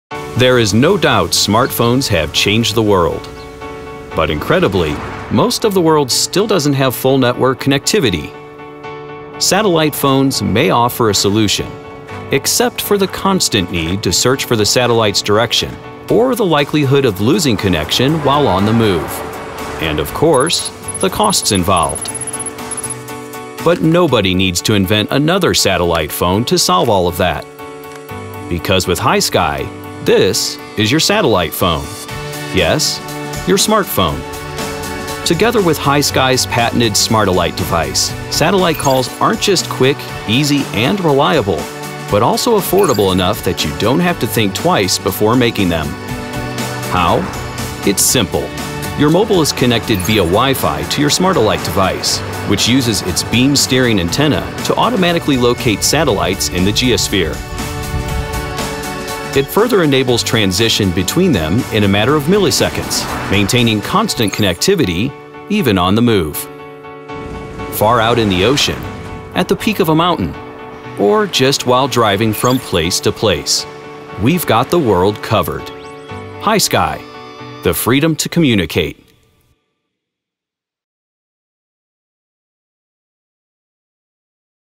hiSky 2017
hiSky introduces the Smartellite, its revolutionary Ka-band portable satellite terminal for affordable satellite calls and IOT.